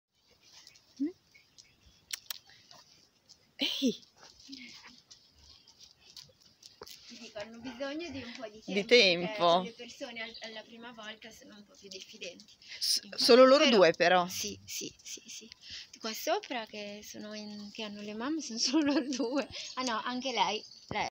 Pastore Belga femmine in regalo
Soyala simil pastore belga , un manto nero corvino e un fascino lupino il suo , una cagnolina dall'aspetto elegante che lascia a bocca aperta . Una meravigliosa lupetta di taglia media contenuta (15kg) , molto giovane ha solo 1 anno e mezzo !!! Di carattere leggermente timida con chi non conosce ma le basta poco per prendere fiducia e lasciarsi andare . Bisogna proprio dare una possibilità a questa creatura meraviglia incrocio con pastore belga . Si trova vicino Rimini , adottabile al centro e nord italia . Viene affidata previo colloquio di pre affido già vaccinata , sverminata , sterilizzata e con microchip .